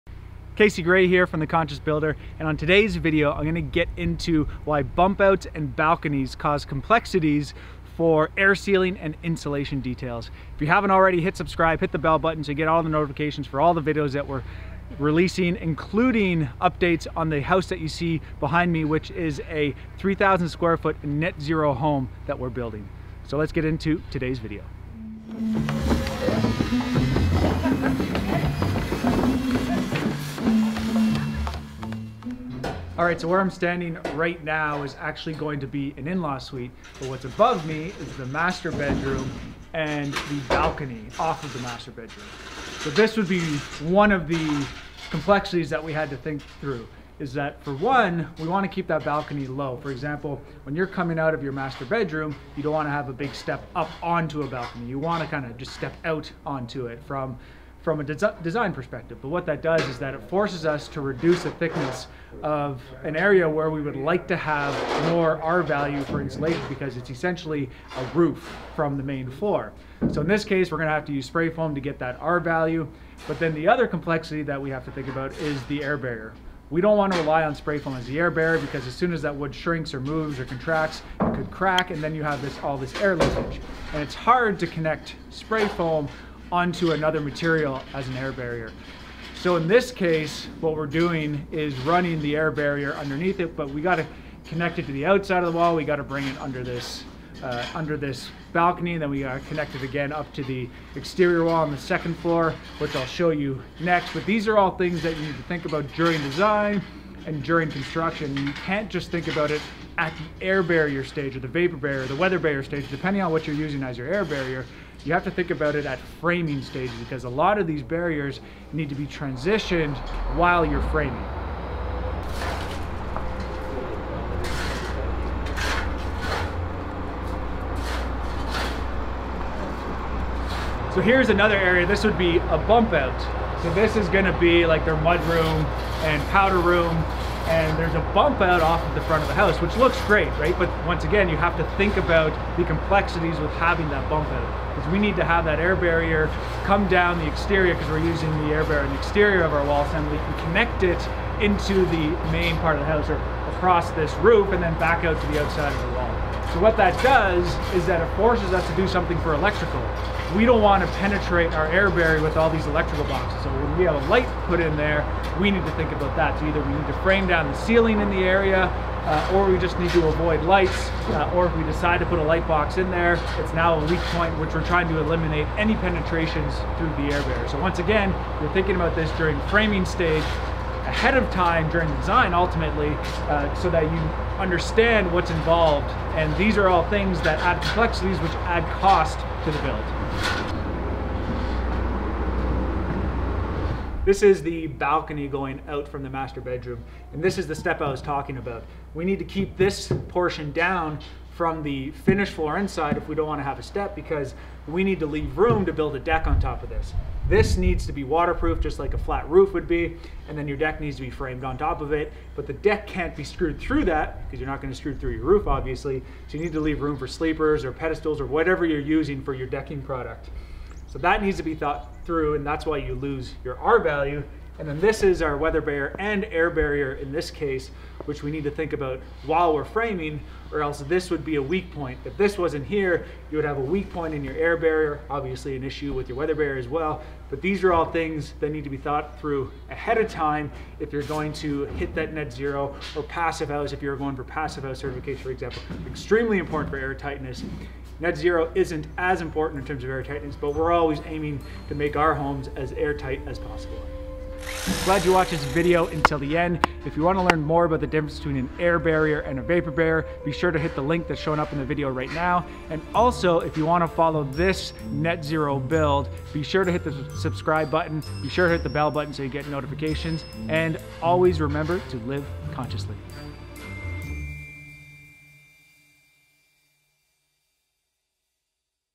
How Balconies & Bump Outs Add Complexity and Affect your Air Barrier | Ask Casey Grey
Welcome to another episode of Ask Casey Grey. This week he dives into why bump outs and balconies cause complexities for air sealing and insulation details.